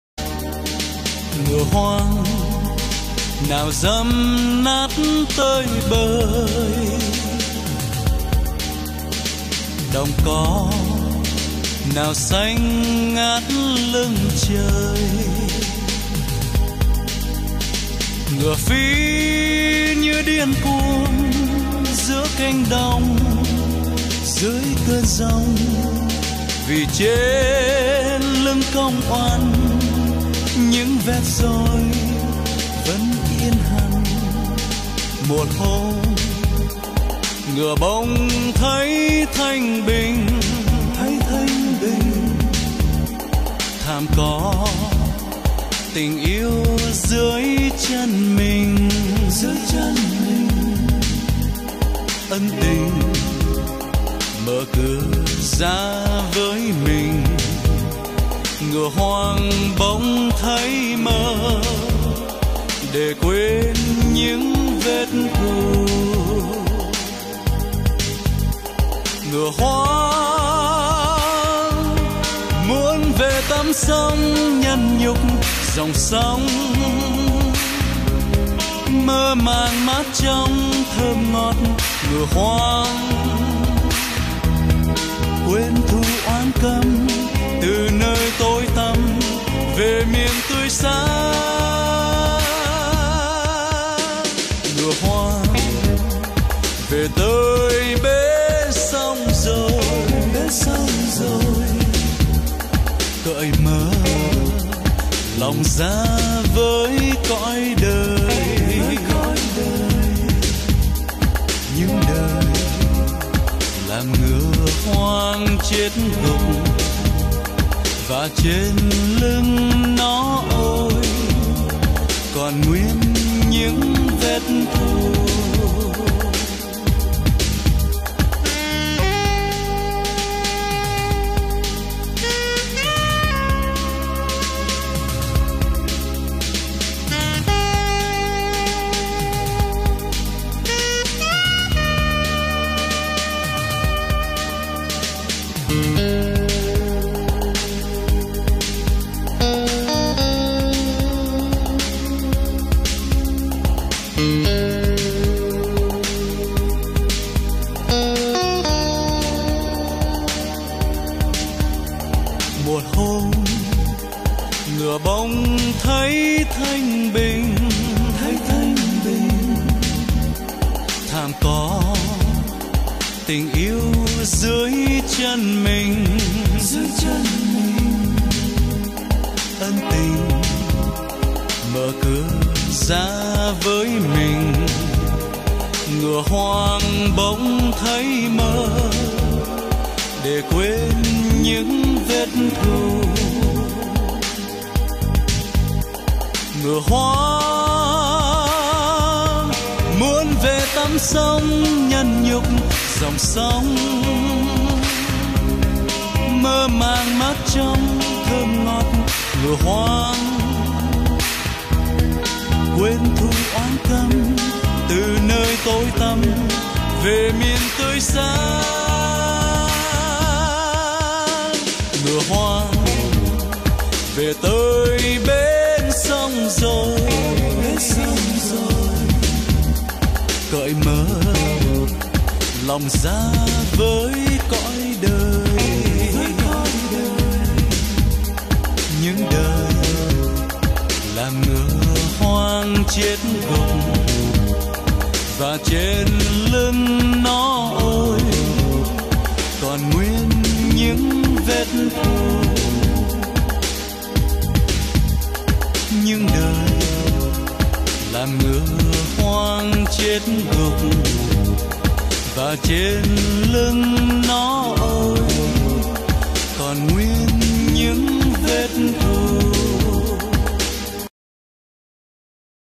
Vết Thù Trên Lưng Ngựa Hoang - 2014
Vet Thu Tren Lung Ngua Hoang 2014, toan la nu tuong trong Cau Lac Bo Hung Su Viet dang tap tran de chuan bi cho phim moi. Hy vong  cong  quy vi, Co le se duoc phat hanh trong tuong lai..Moi coi cho vui